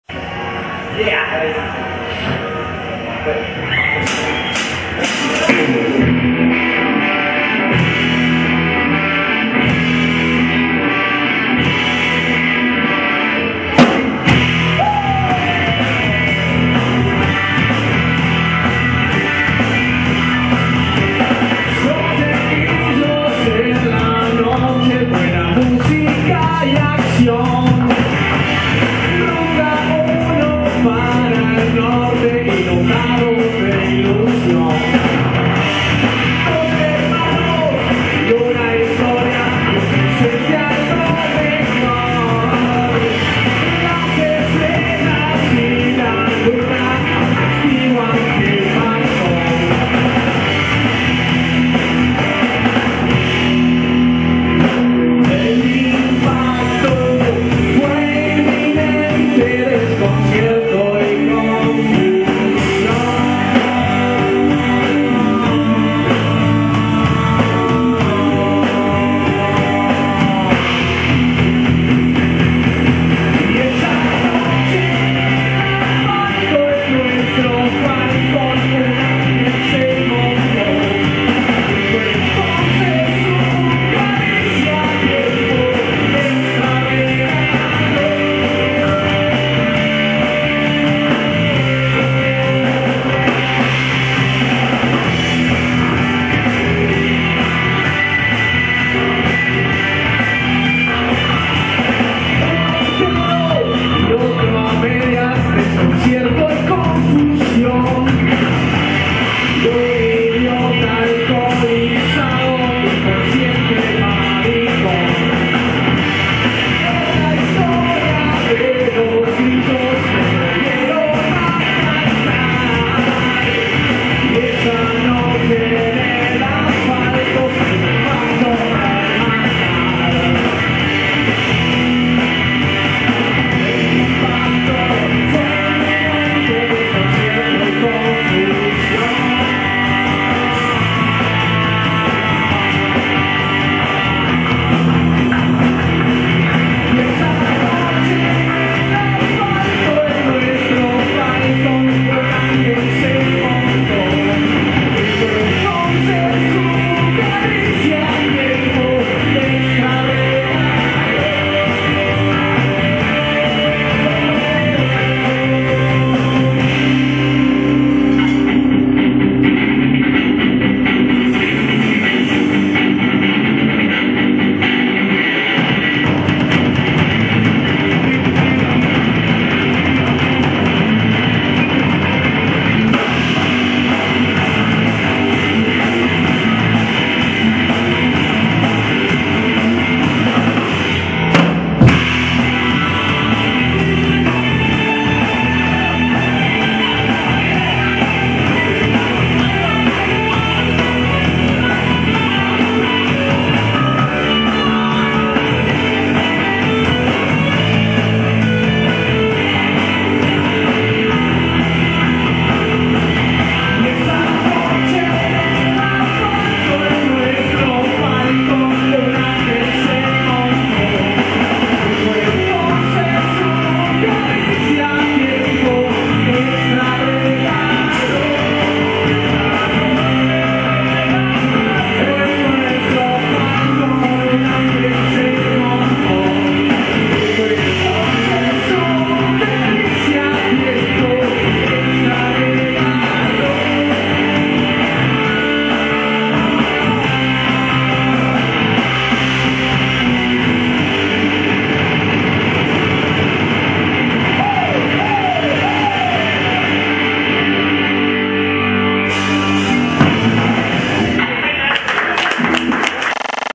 Capital Rock
la banda presentó su disco en un bar de la capital santafesina